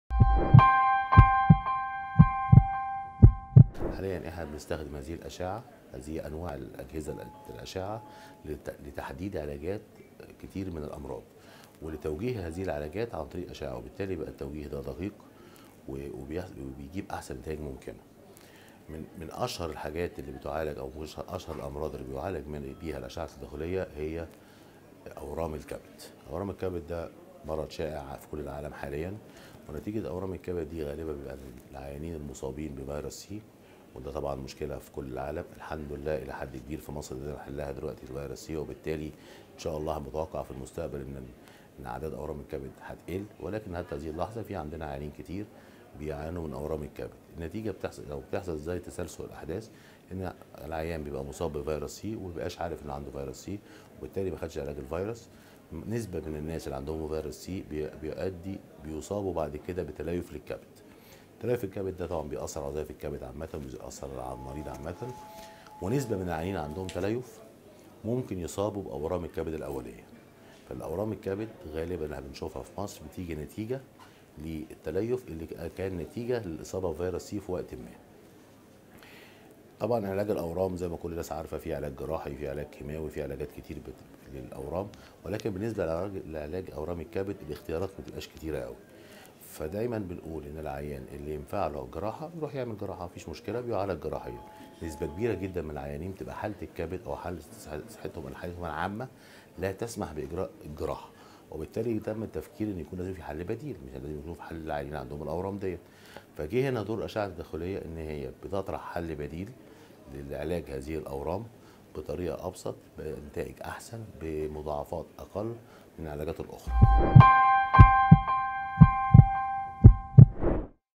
خالد شحاتة: مرضى فيروس "سي" معرضون للإصابة بأورام الكبد
الدكتور خالد شحاتة، استشاري الأشعة التداخلية بمستشفى كليوباترا، يتحدث عن توجيه العلاجات من خلال الأشعة التداخلية، وأسباب الإصابة بأورام الكبد.
تقرير: مصطفي عريشة
تصوير: يمني بدر